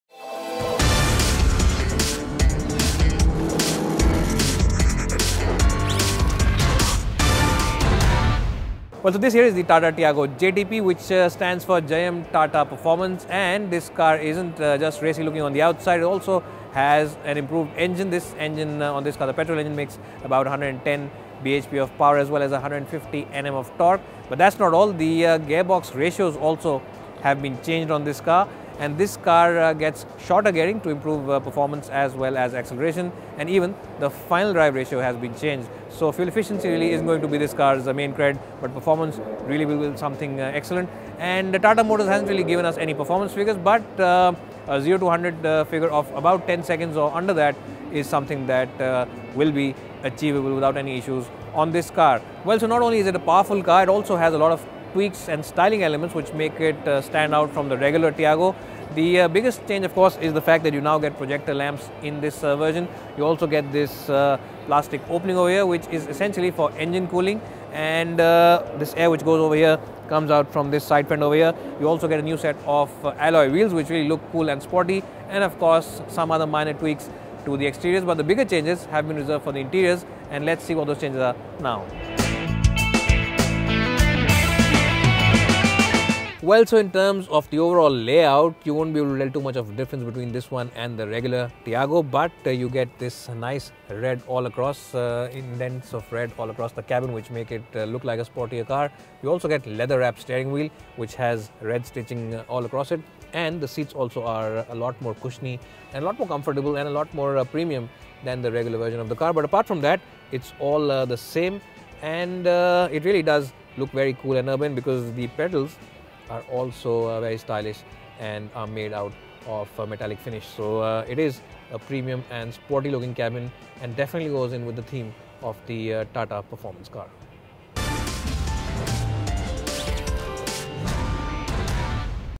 Tata Tiago JTP at Auto Expo 2018 - Autoportal
This car looks much sportier from the outside than the regular Tiago. The improved engine on this car makes a power of 110bhp with 150 Nm of Torque. The gearbox ratios have also been changed from inside. The fuel efficiency would be the main feature of this car. Projector lamps are available in this version with a plastic opening for engine cooling. Let's find out more